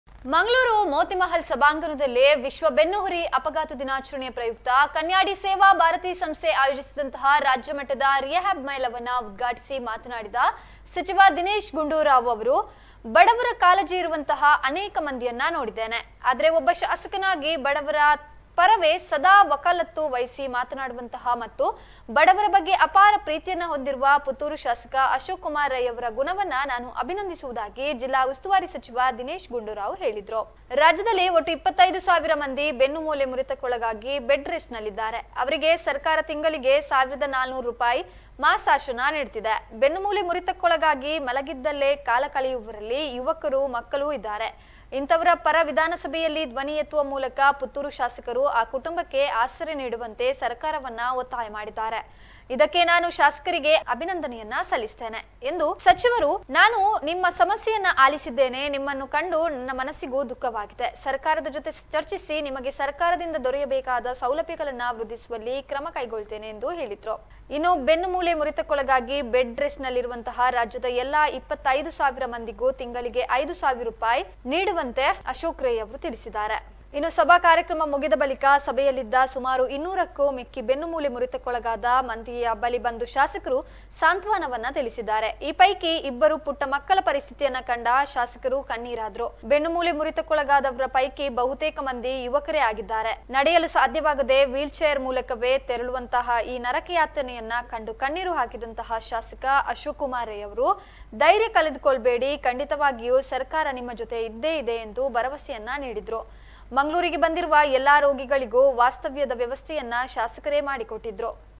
ವಿಶ್ವ ಬೆನ್ನುಹುರಿ ಅಪಘಾತ ದಿನಾಚರಣೆ
ರಾಜ್ಯ ಮಟ್ಟದ ರಿಹ್ಯಾಬ್ ಮೇಳ
ಶಾಸಕ ಅಶೋಕ್ ರೈ ಯವರ ಬಡವರ ಮೇಲಿನ ಪ್ರೀತಿ ಅಪಾರ: ದಿನೇಶ್ ಗುಂಡೂರಾವ್